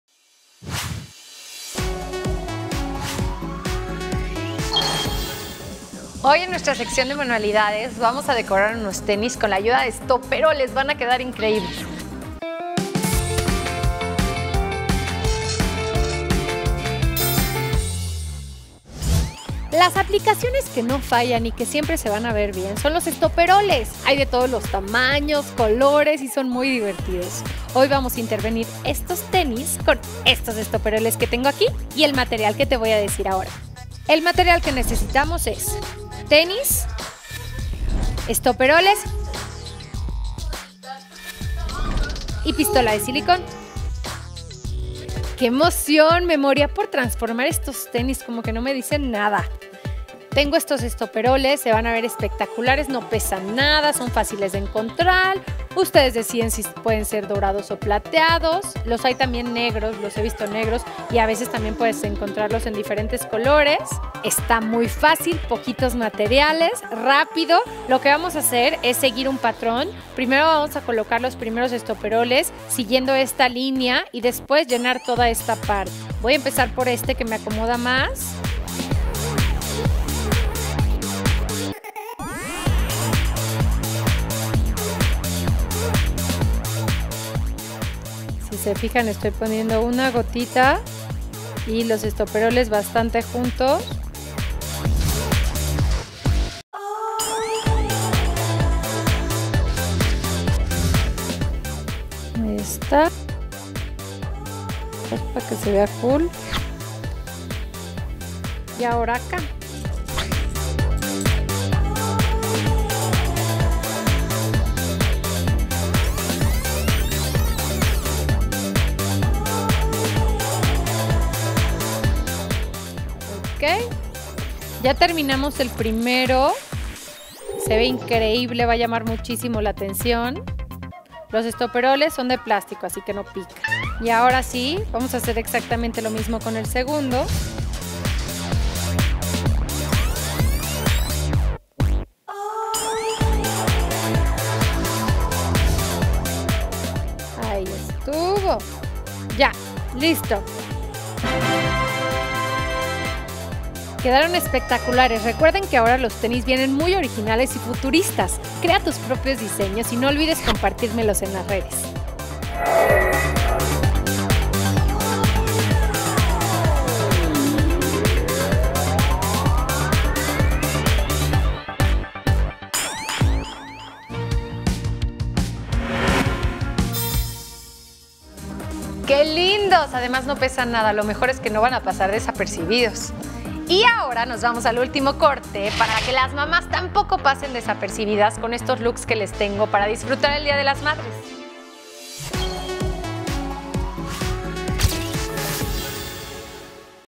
¡Decora tus tenis con estoperoles! / Dress Code Descripción:
Chantal nos ayuda a poner aplicaciones en nuestros tenis de diferentes tamaños y colores para que se vean más divertidos.